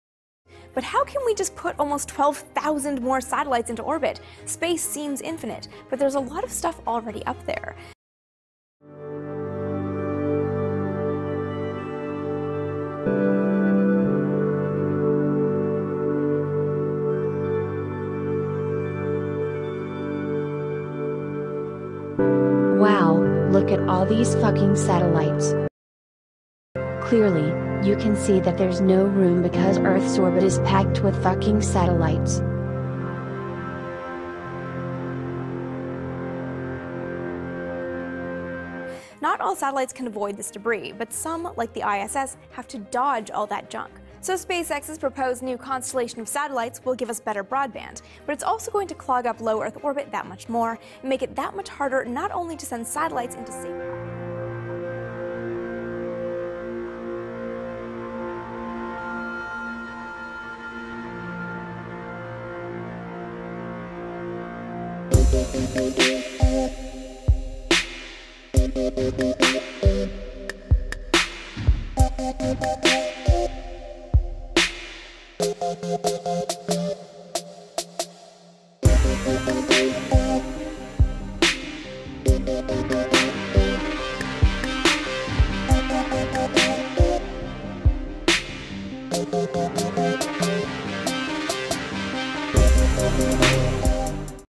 Flat Earth: There's Already a lot of Stuff Up There
Absolutely thick with satellites but they're kinda hard to see I guess